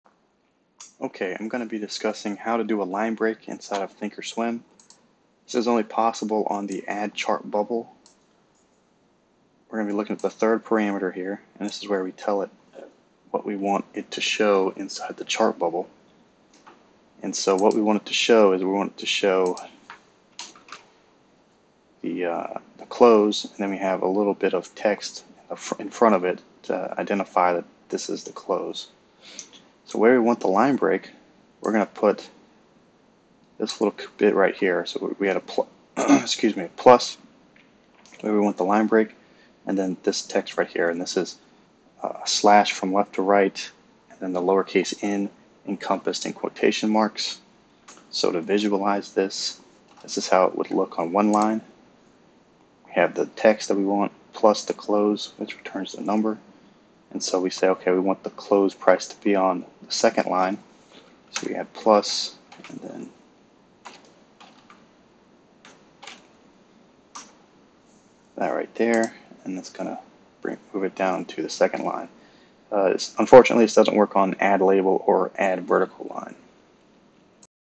How to Add a Line Break to Chart Bubbles Inside ThinkOrSwim
Quick video to show how to add a line break to a chart bubble inside thinkorswim